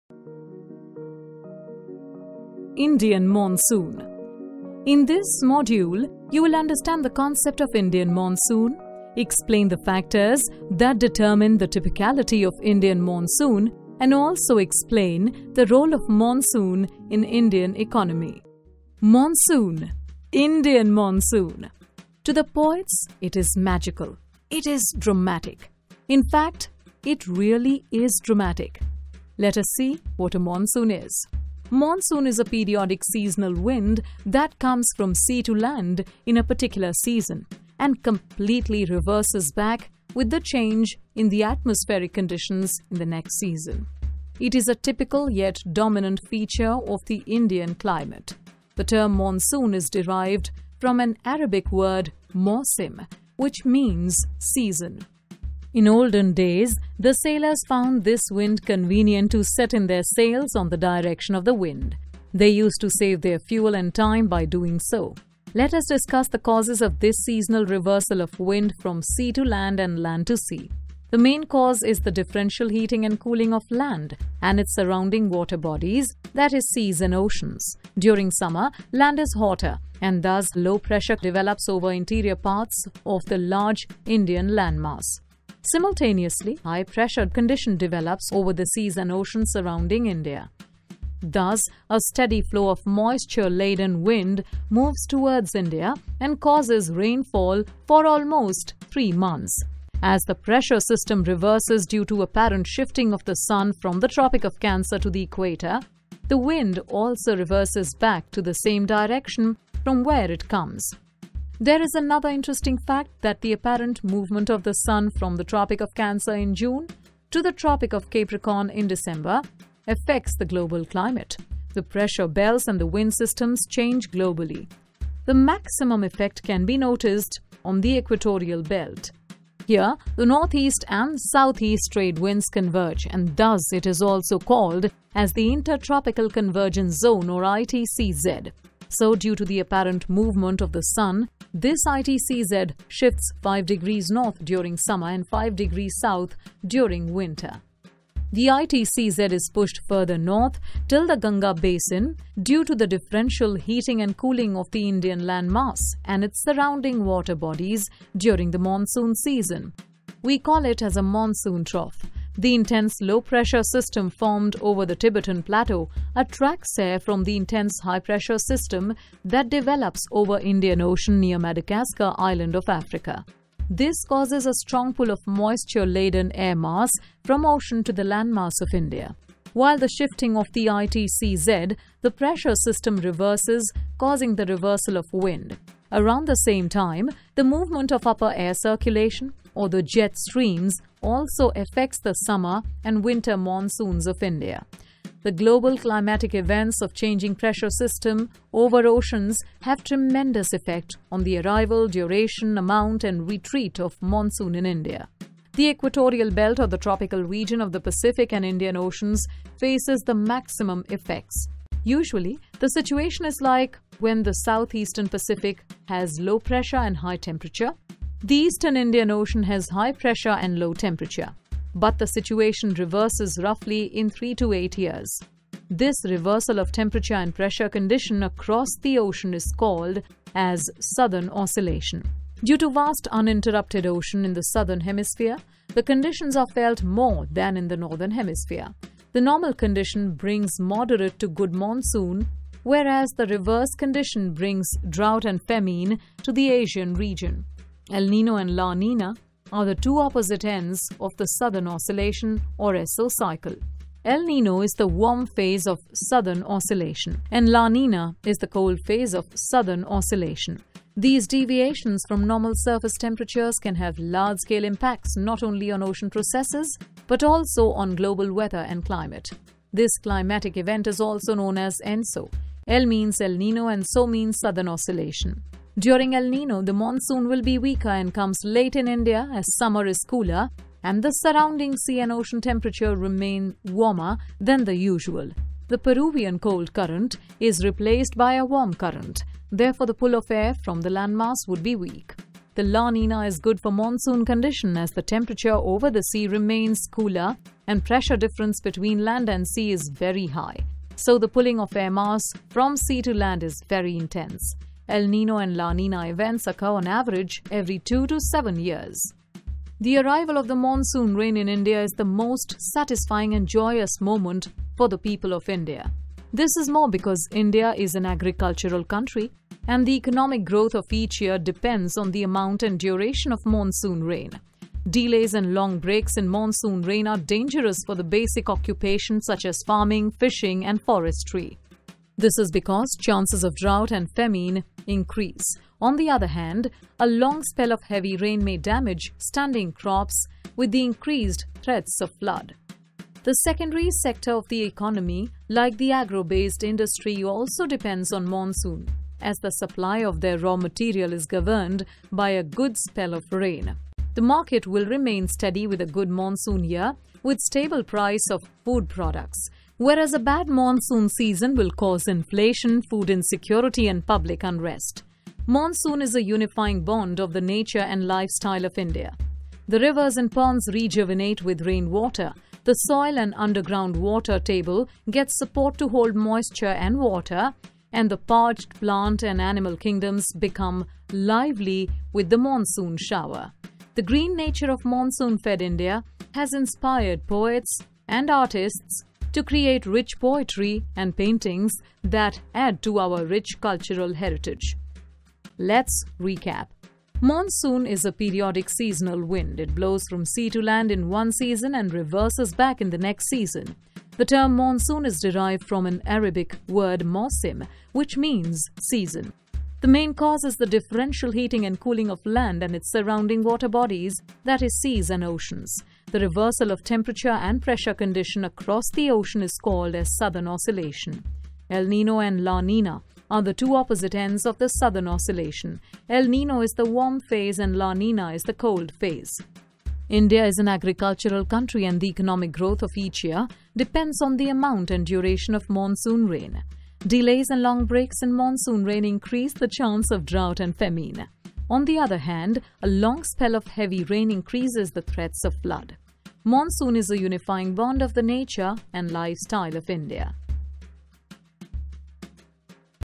Indian Monsoon Class 9 Geography | NCERT Chapter 4 Climate Full Explanation
Indian Monsoon | Class 9 Geography Chapter 4 - Climate | NCERT Explanation

In this module, you will:
✅ Understand the concept of the Indian monsoon
✅ Explore the factors that make the Indian monsoon unique
✅ Learn how the monsoon impacts the Indian economy

The Indian monsoon is a periodic seasonal wind that plays a vital role in shaping India’s climate and economy. It blows from sea to land in one season and reverses in the next.

🔹 The word Monsoon comes from the Arabic word 'Mausim', meaning season.
🔹 It is caused by the differential heating of land and water bodies.
🔹 The Southern Oscillation is a key phenomenon involving pressure changes across oceans.
🔹 El Niño (warm phase) and La Niña (cold phase) are two contrasting effects of this oscillation.

🌾 As an agriculture-based nation, India's economy heavily depends on the timing, duration, and strength of the monsoon rains each year.

📚 Ideal for: Class 9 students | CBSE | NCERT Geography | Competitive Exam Prep